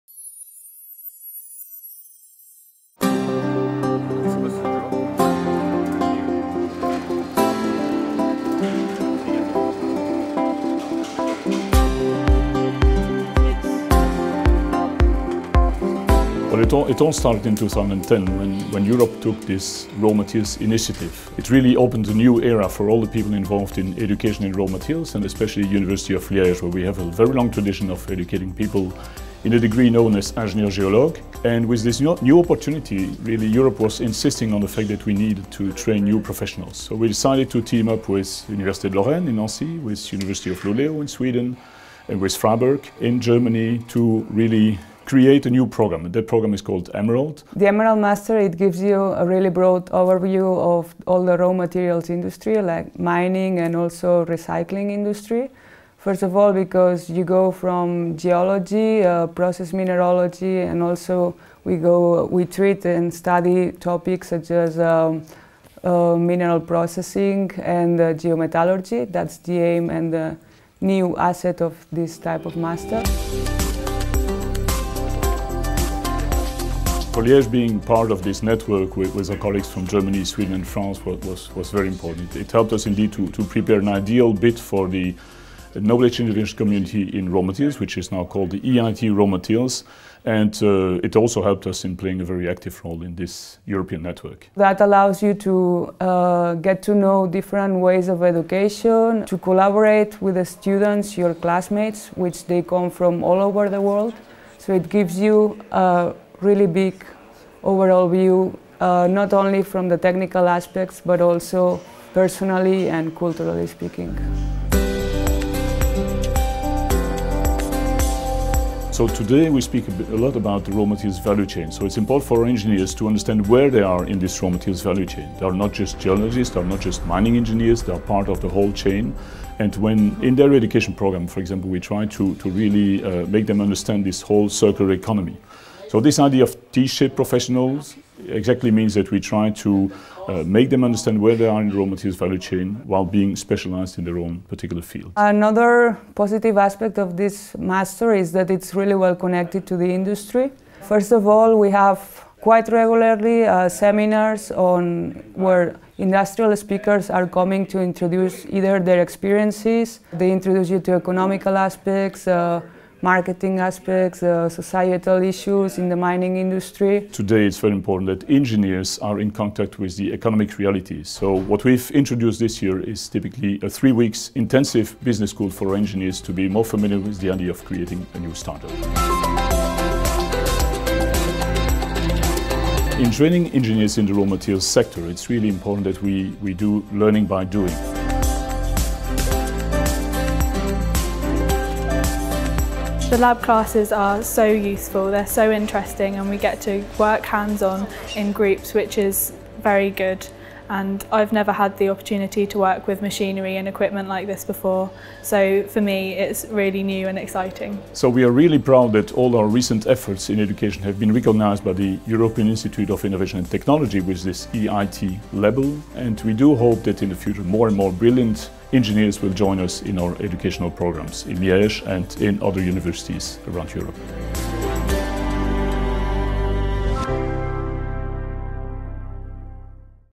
EMERALD - Master course in Georesources Engineering
EMerald at University of Liège is a two-year master program designed to focus on the following major aspects of georesources engineering : Characterisation, Processing and Modelling.